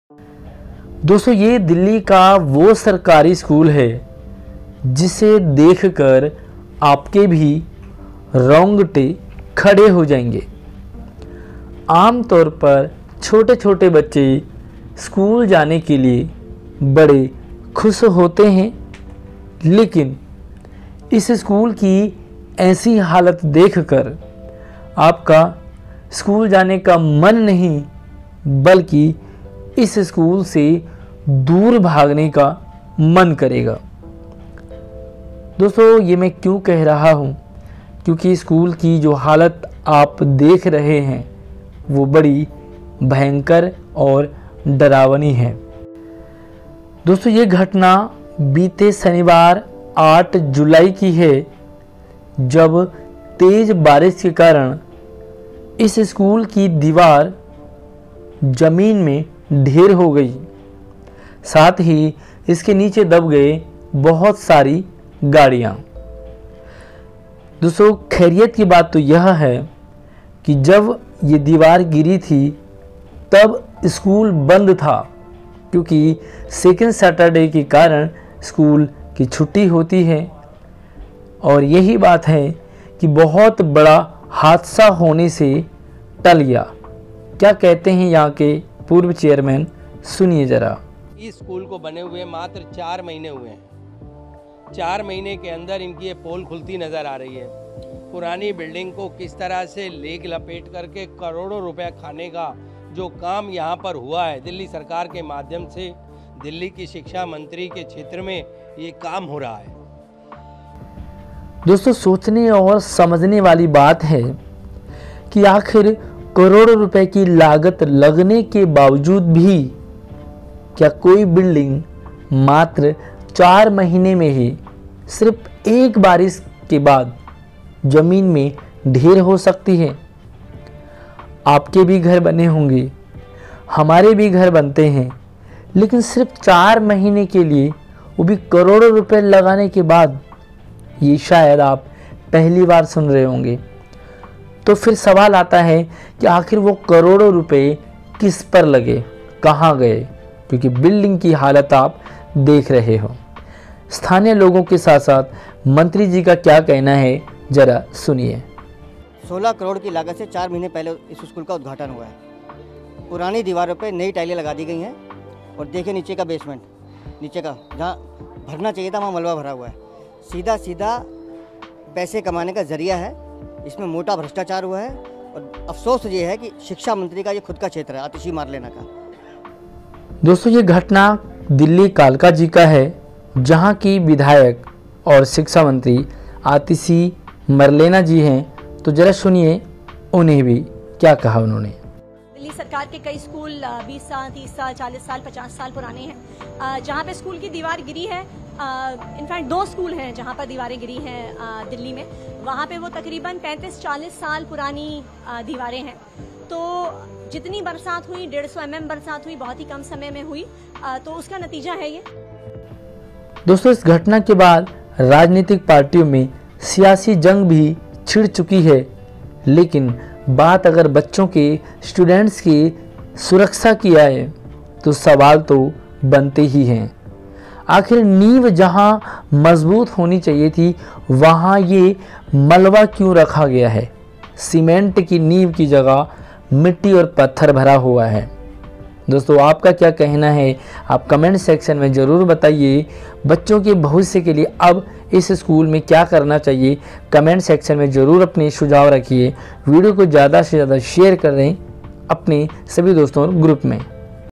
OMG ये दिल्ली सरकारी स्कूलों को क्या हो गया?? स्टूडेंट्स अपने आप को स्कूल में कैसे सुरक्षित समझेंगे😳
OMG ये दिल्ली के सरकारी स्कूलों को क्या हो रहा है?? स्टूडेंट्स अपने आप को स्कूल में कैसे सुरक्षित महसूस करेंगे।।। पूरी वीडियो देखकर समझे पूरा मामला। साथ ही वीडियो को ज्यादा से शेयर करते हुए अपना कीमती सुझाव जरूर रखे। धन्यवाद।।